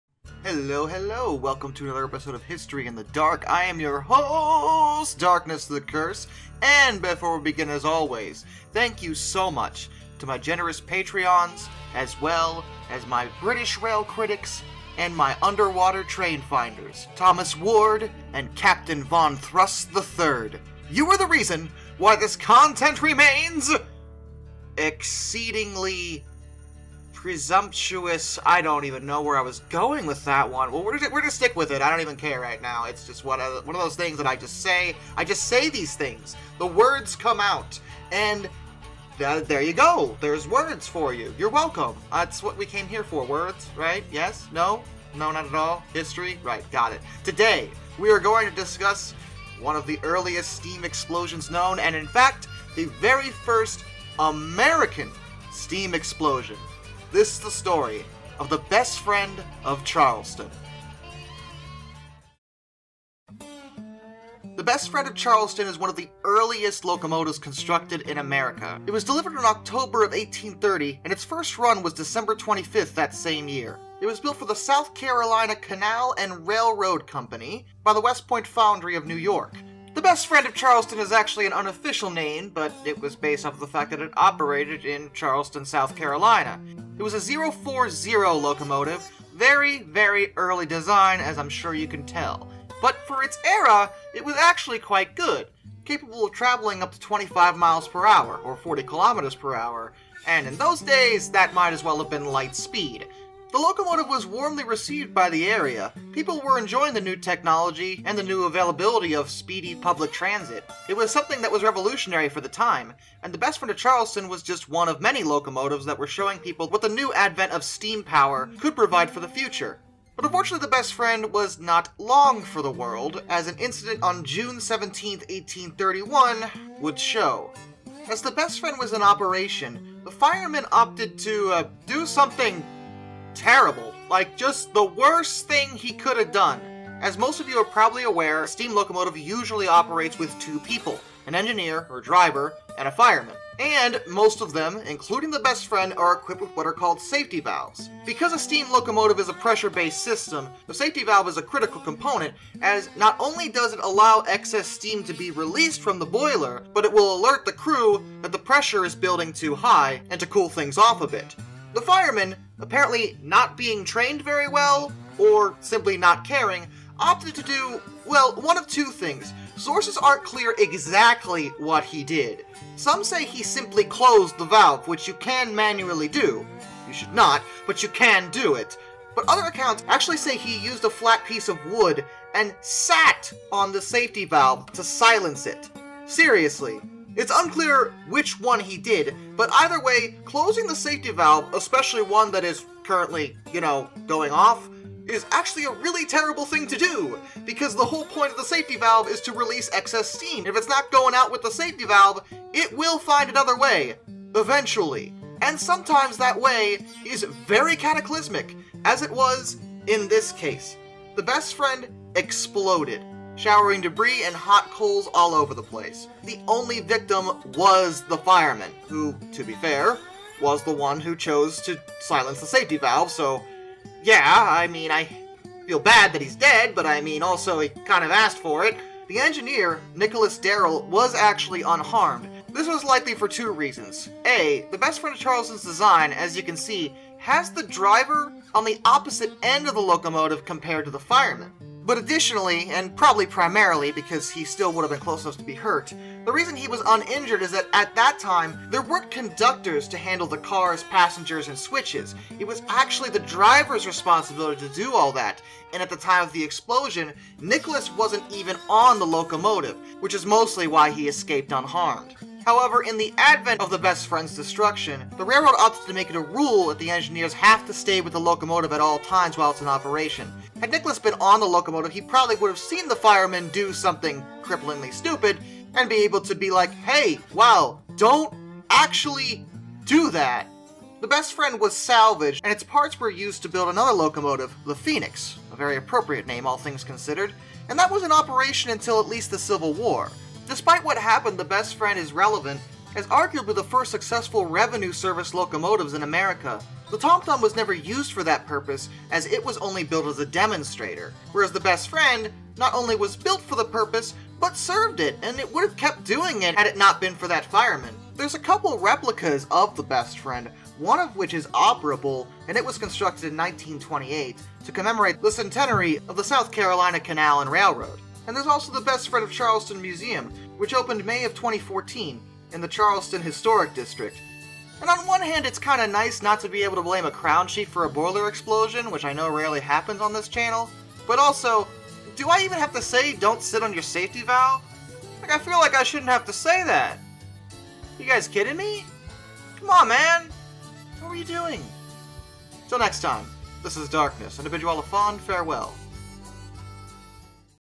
The Explosion of an Early Steam Locomotive 🚂 The Best Friend of Charleston 🚂 History in the Dark
The Best Friend of Charleston was the first American steam locomotive built for revenue service. It was successful in its duties, until a careless fireman caused its untimely destruction.